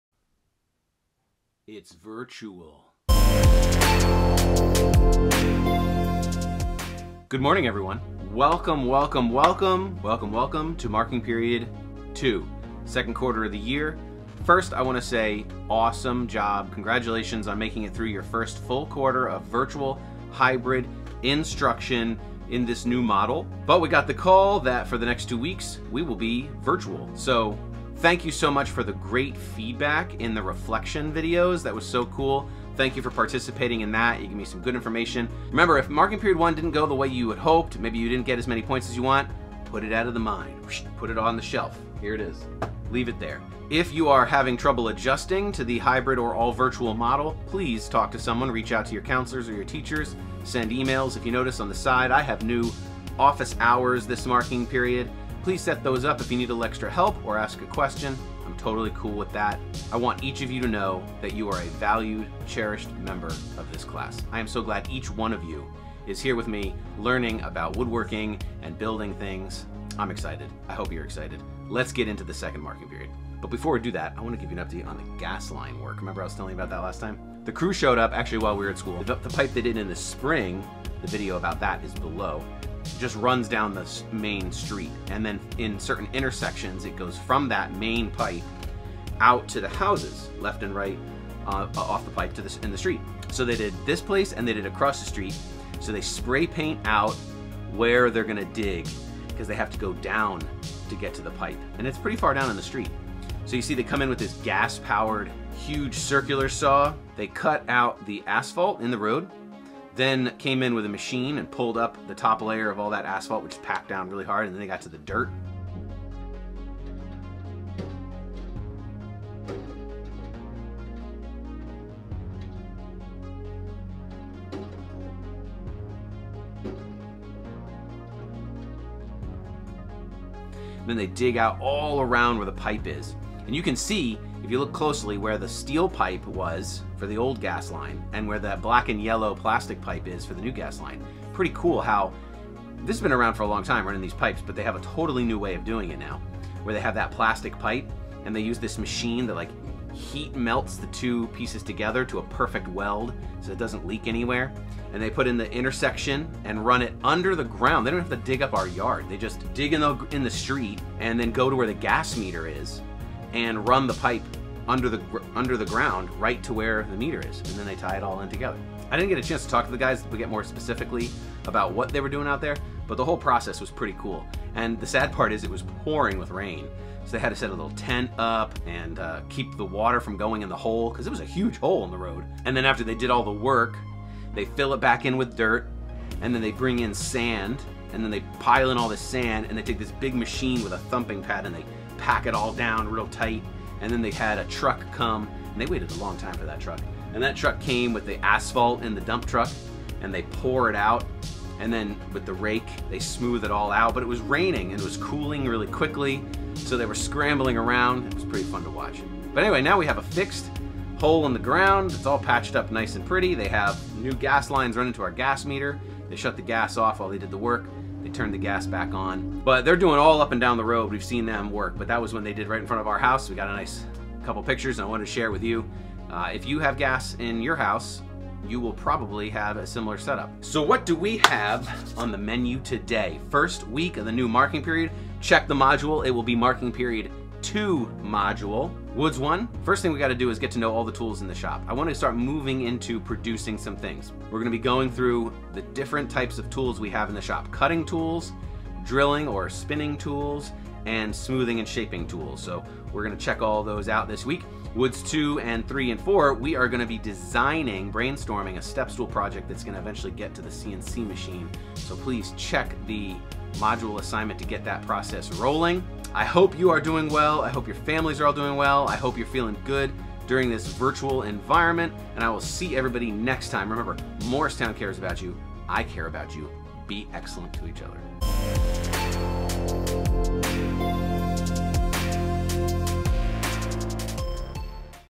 We got a new gas line! - VLOG #66
Good Morning November 16, 2020
First day of the second marking period. Gas company replaced the gas line to our house.
This video is a part of a series that I produced for students in my Woods classes during the 2020/2021 school year.